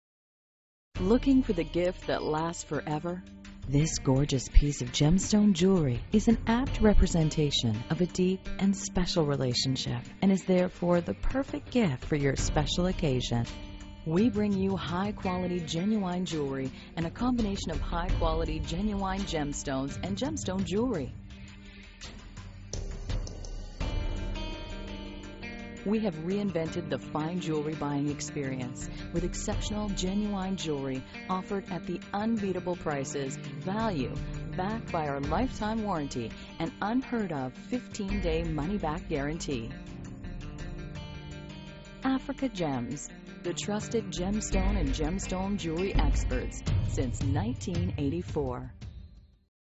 Lively and Lovely Star Burst Genuine Alexandrite Flower Pendant With Diamond Accents - 0.51 carats -
This Lively and Lovely Star Burst Genuine Alexandrite Flower Pendant With Diamond Accents - 0.51 carats is brought you - The Trusted Gemstone Experts Since 1984.